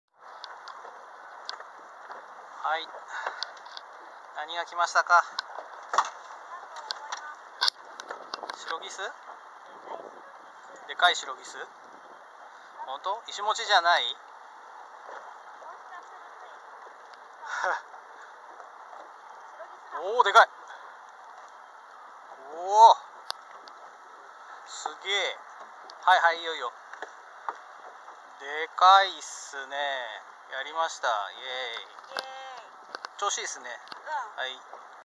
200614 シースタイル シロギス その２
今回は東神奈川のＤ－ｍａｒｉｎａさんから２回目のシースタイル利用です！狙いはシロギス＆アジ！